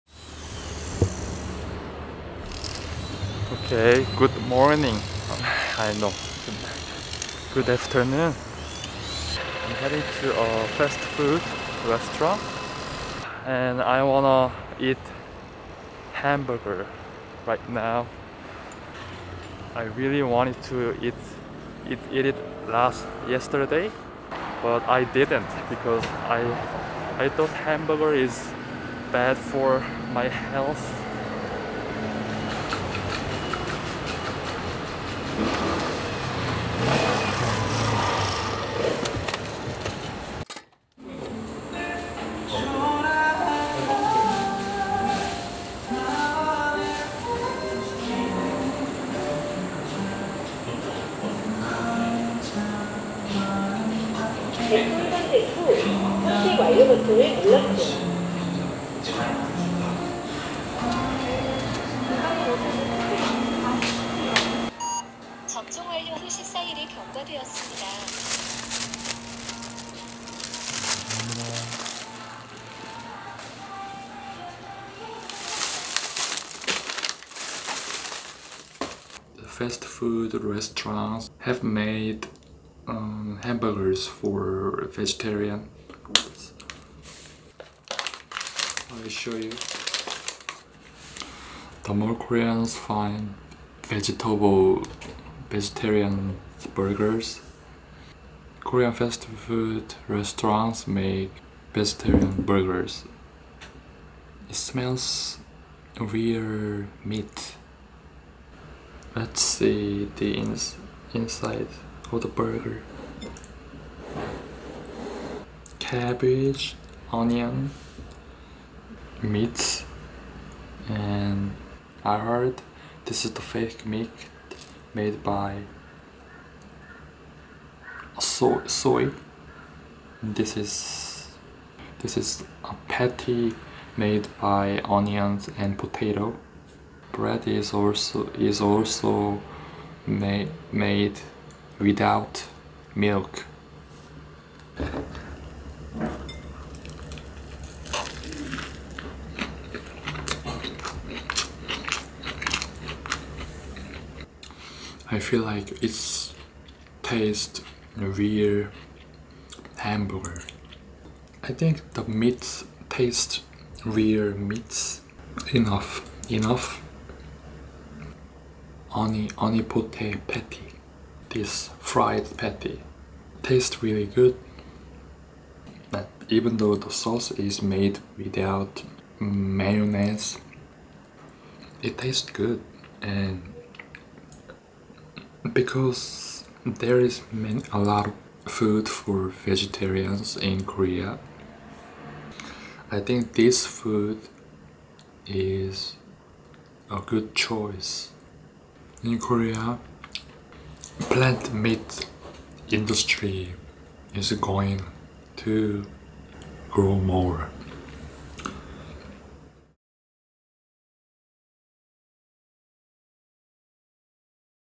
More veggie burgers are being made in Korea.
As I got interested in health and environmental pollution, I tried a veggie burger once.